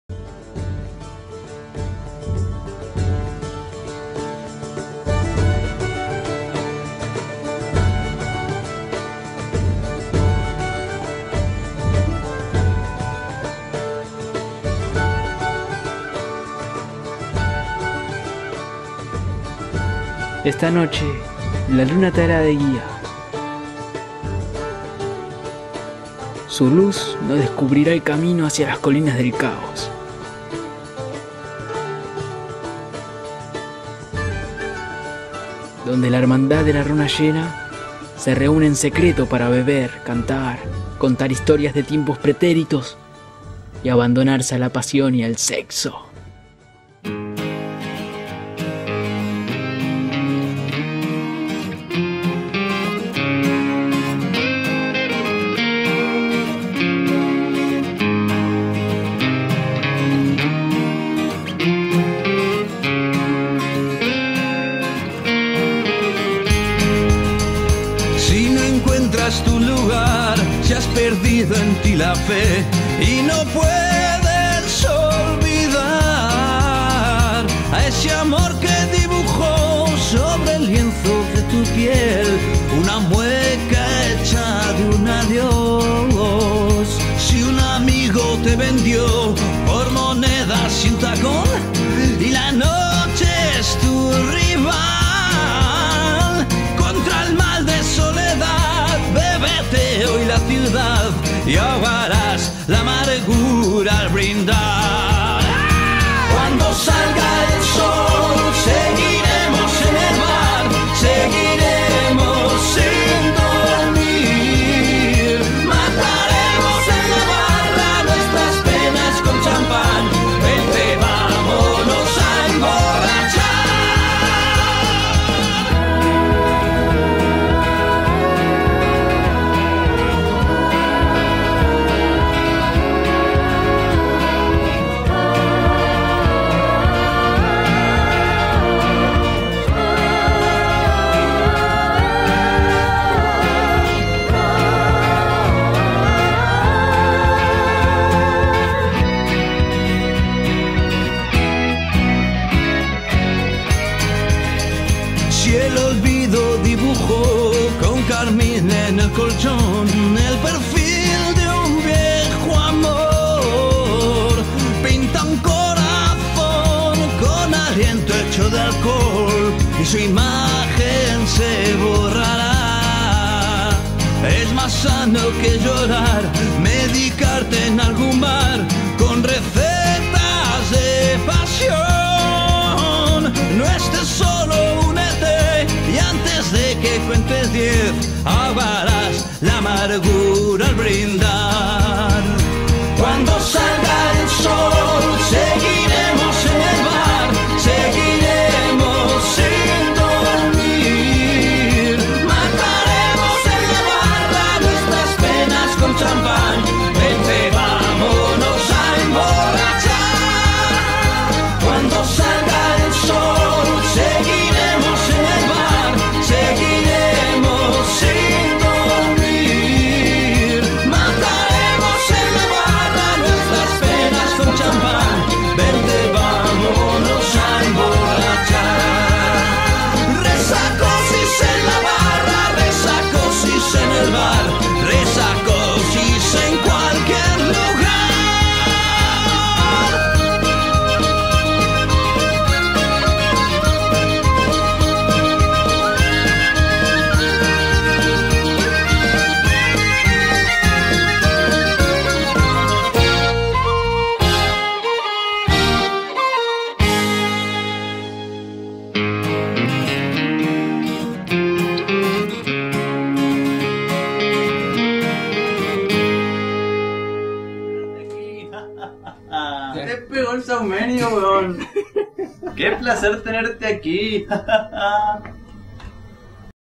La mágica ciudad de los árboles - Capítulo 9 - Resacosix en la barra
Pero esa noche la luna también iba a hacerme de guía, como si diera manotazos a una nube que la medio ocultaba, se abrió paso a puñetazos dejando trozos de nubes a su alrededor. Al verse libre de ese molesto velo, se asomó y dejó escapar guiños de luz descubriéndome el camino hacia las colinas del kaos. Allí contemplé atónito una reunión secreta de la hermandad de la runa llena...